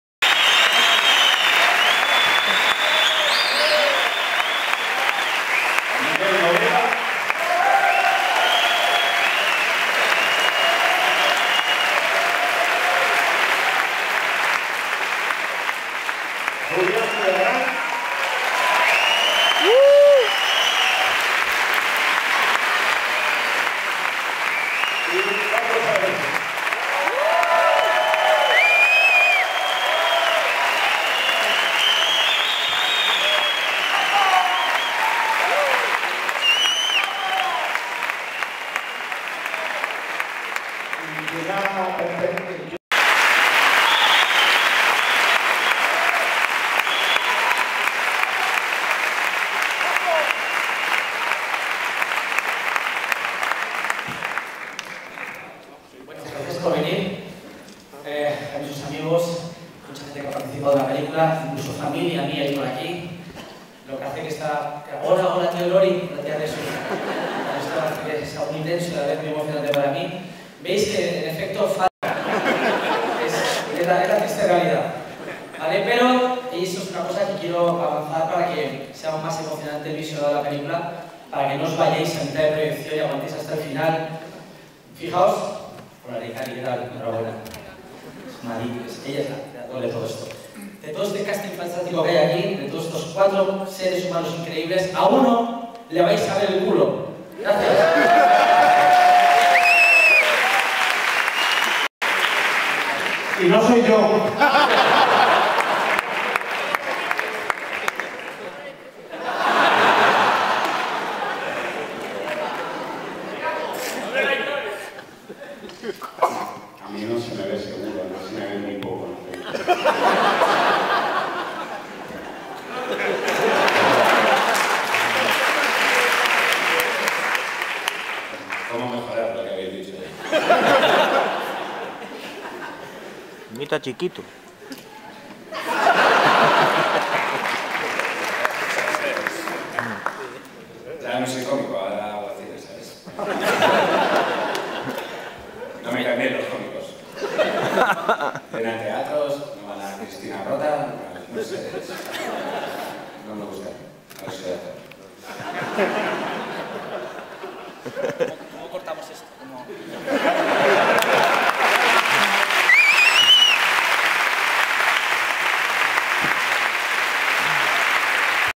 Zinemaldia 2011 - Extraterrestre (Presentación)
Presentación de la película "Extraterrestre" de Nacho Vigalondo. Junto a él están la productora de la película y los actores Raúl Cimas, Miguel Noguera, Carlos Areces y Julián Villagrán. La peli se presentó en la sección Zabaltegi - Especiales del Festival Internacional de Cine de San Sebastián - Zinemaldia 2011.